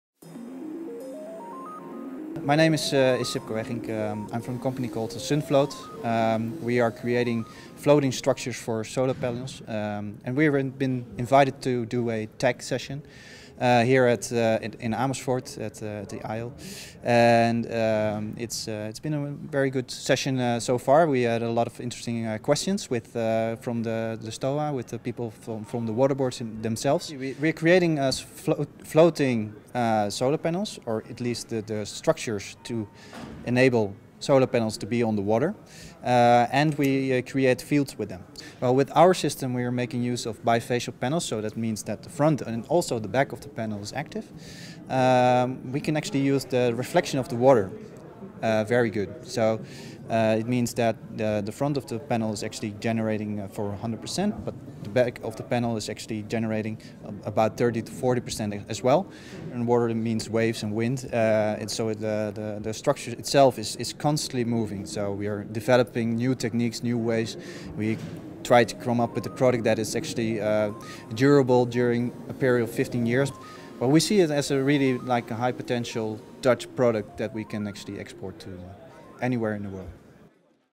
Themadag Greendeal 2017 | Groene Waterschappen | Sunfloat - Sipco Eggink (ENG)
Interview opgenomen op de bijeenkomst Innovatieve technologieën voor groene waterschappen (rapport ) op donderdag 20 april 2017 - De Observant te Amersfoort. Meer informatie